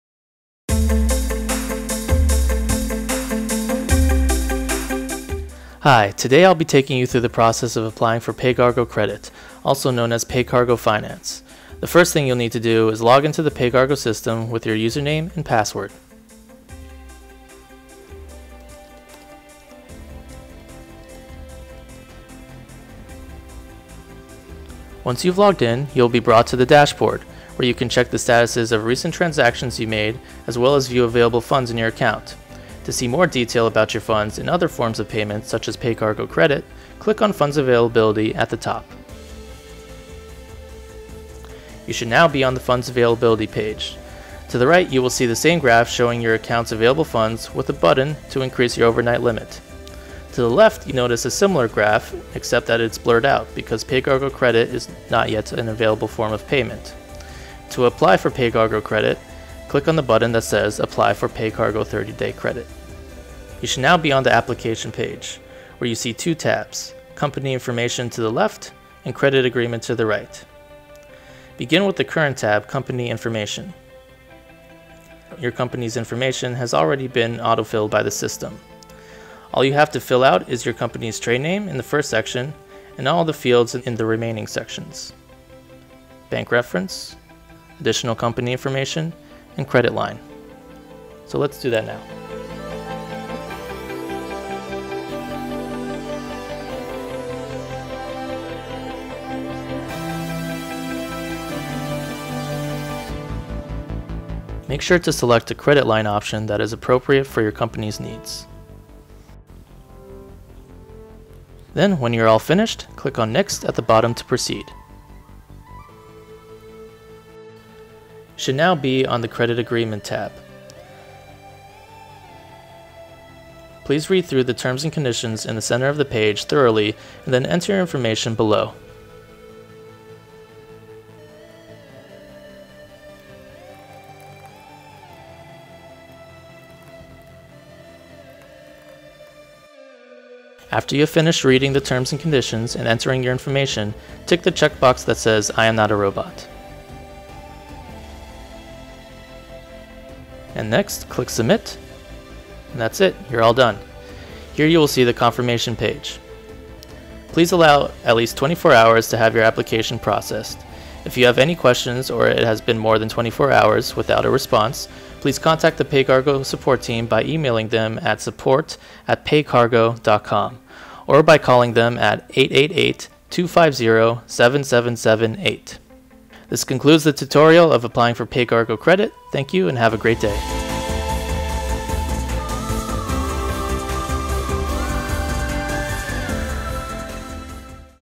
Applying for PayCargo Credit
This video will show you how to apply for PayCargo Credit as a Payer in the PayCargo system.

PayCargo.com